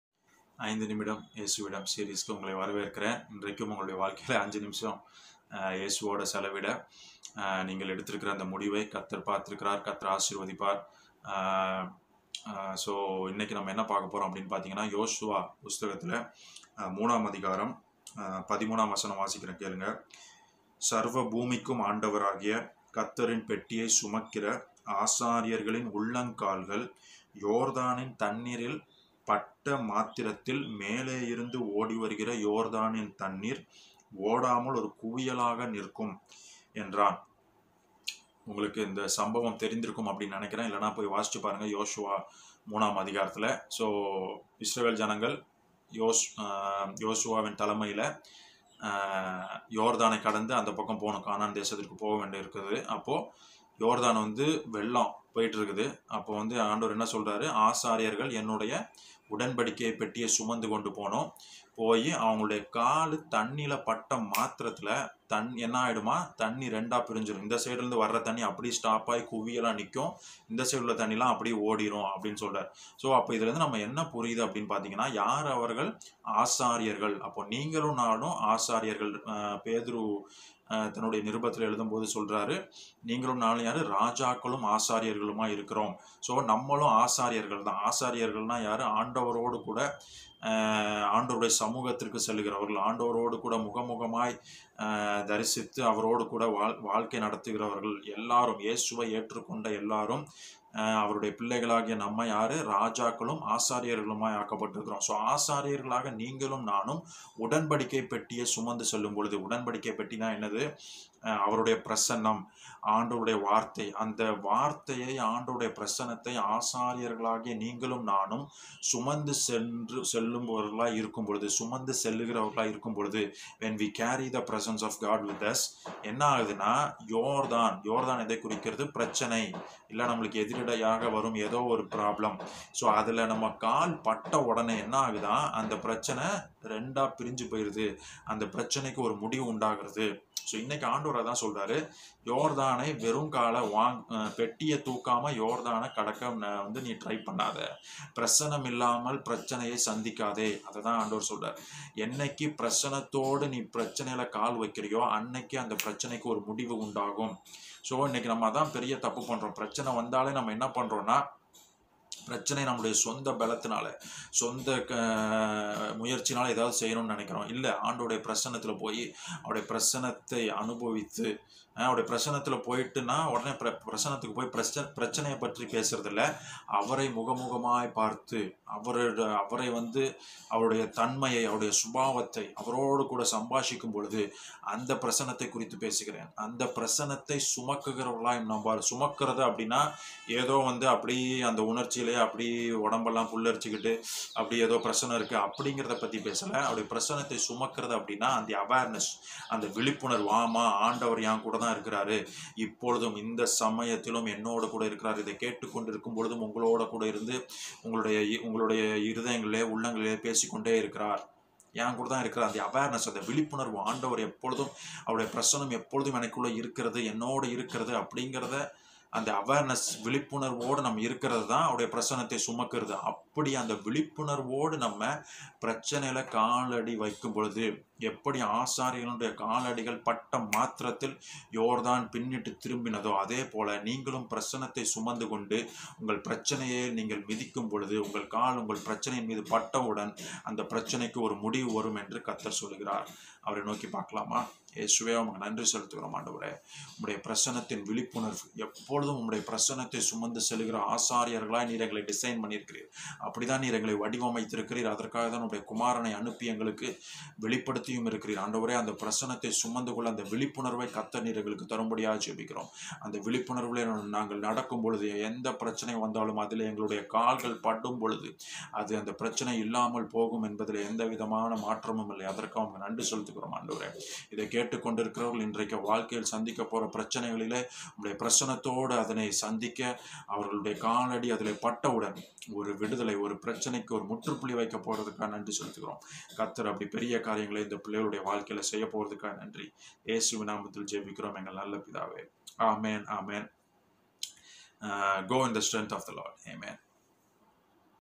ஐந்து நிமிடம் இயேசுவிடம் | பாதம் பட்டவுடன்
ஆண்டவருடைய பிரசன்னத்தை சுமந்து செல்லும் நமக்கு பிரச்சனை ஏற்பட்டால் என்ன நடக்கும்?

List of videos suggesting the above video FOR CHANNEL OWNERS PURPOSE ONLY

தவெக 2 வது  ஆண்டு துவக்க  விழா-ரிசார்ட்டில் ஏற்பாடுகள் தயார்...!